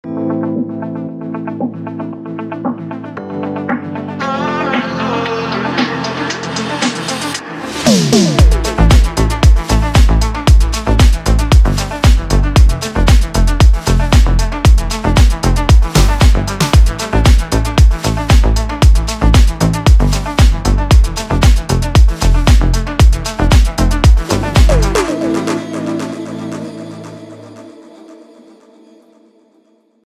ALERTE INNOVATION - ciklo - le premier stylo québécois est enfin là !
1er stylo sourcé, conçu et fabriqué au Québec à partir de matière recyclée et recyclable.